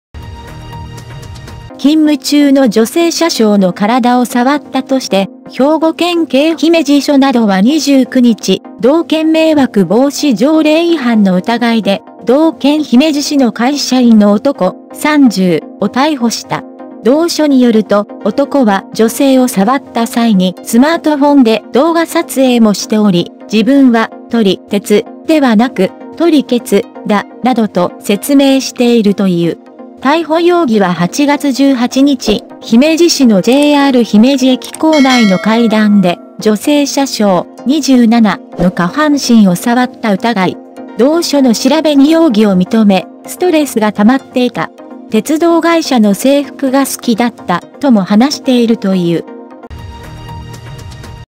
兵庫県立駅でセクハラをした30歳の男性が逮捕された
勤務中の女性車掌の体を触ったとして、兵庫県警姫路署などは２９日、同県迷惑防止条例違反の疑いで、同県姫路市の会社員の男（３０）を逮捕した。同署によると、男は女性を触った際にスマートフォンで動画撮影もしており、「自分は『撮り鉄』ではなく『撮りケツ』だ」などと説明しているという。
逮捕容疑は８月１８日、姫路市のＪＲ姫路駅構内の階段で、女性車掌（２７）の下半身を触った疑い。同署の調べに容疑を認め、「ストレスがたまっていた。鉄道会社の制服が好きだった」とも話しているという。